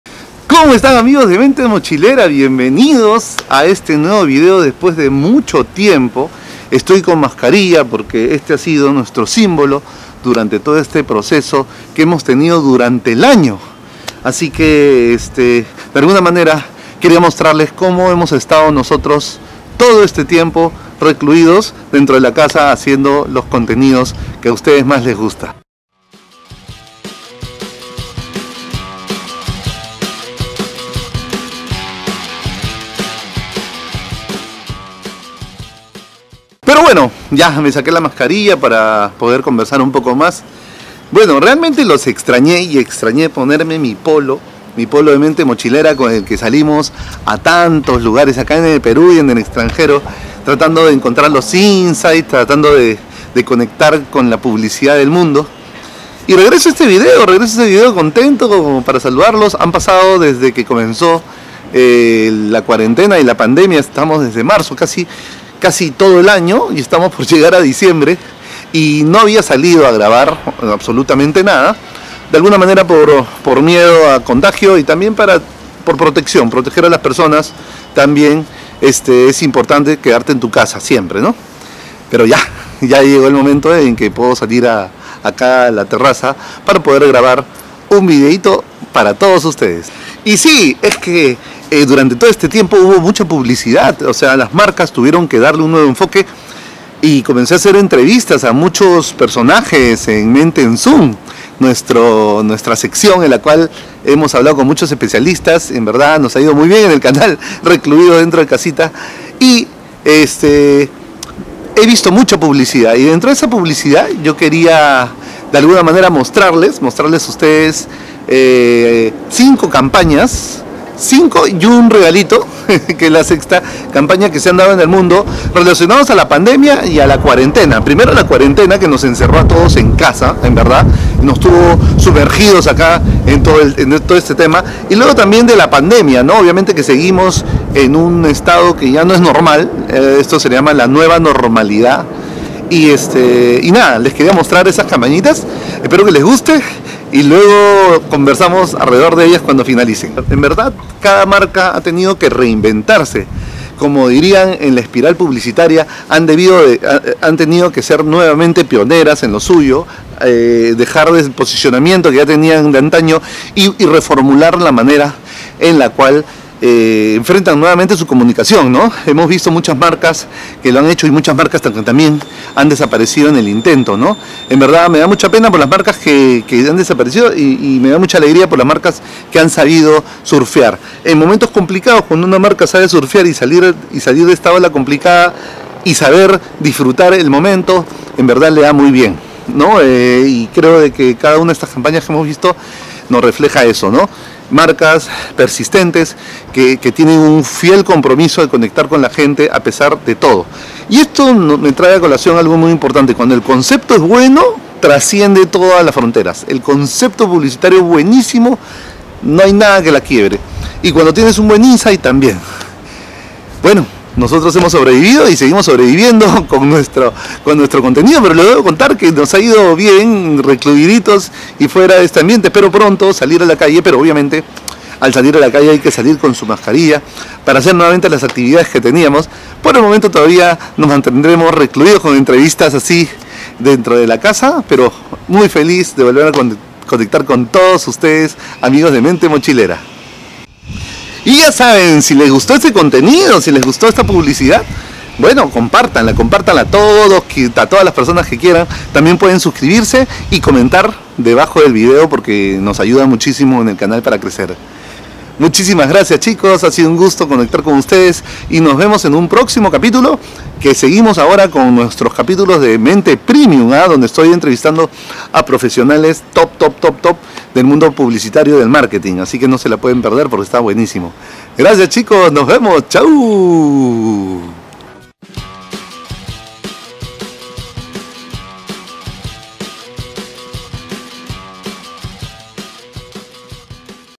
Lo que callé de la Publicidad en PANDEMIA:: SIN FILTROS
Regresamos por un momento al sol para saludarlos y analizar un poco las publicidades que se dieron en pandemia. Cada una de las marcas debió tener una gran estrategia detrás para sacar campañas muy bien enfocadas ya que la nueva realidad hizo que pensemos diferente.

Mor motivos de Copywritte no tuvimos que sacarel video anterior y volver a subir este pero con las escenas recortadas y sin audio, por ello, y para no perder la escencia del video les dejo debajo los links de las campañas completas para que las pueda ver y disfrutar de las campañas. Espero les guste y pronto seguiremos haciendo en vivos, entrevistas y contenido de calidad para todos.